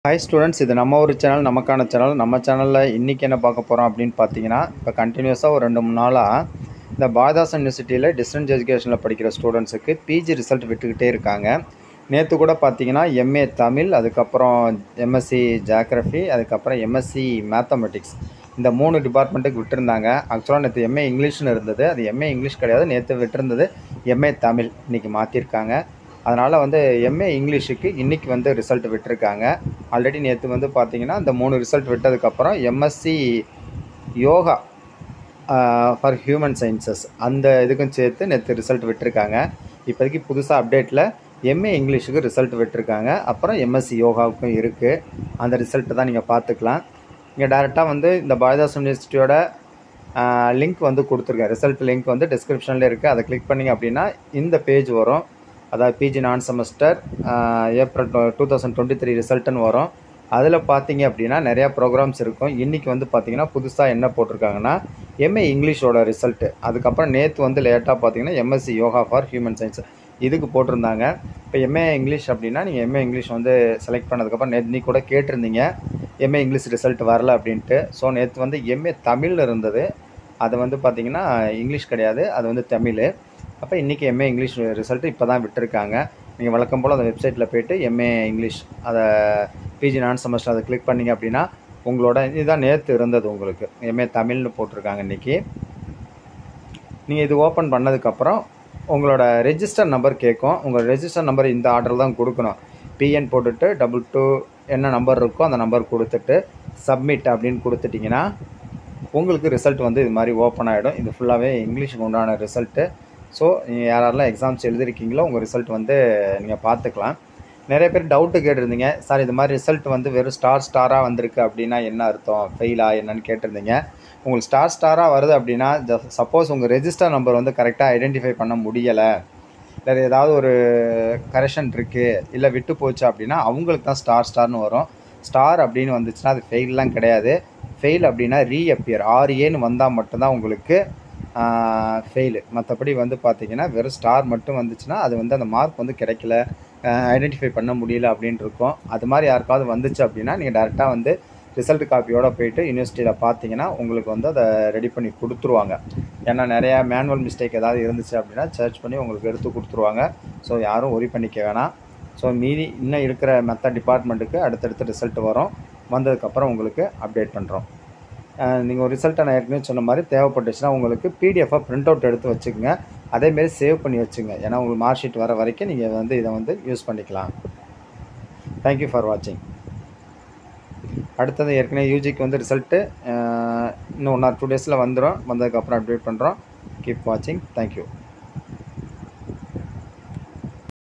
Bharathidasan University Distance Education April 2023 MA English Results Published
Bharathidasan University
 Exam Result Website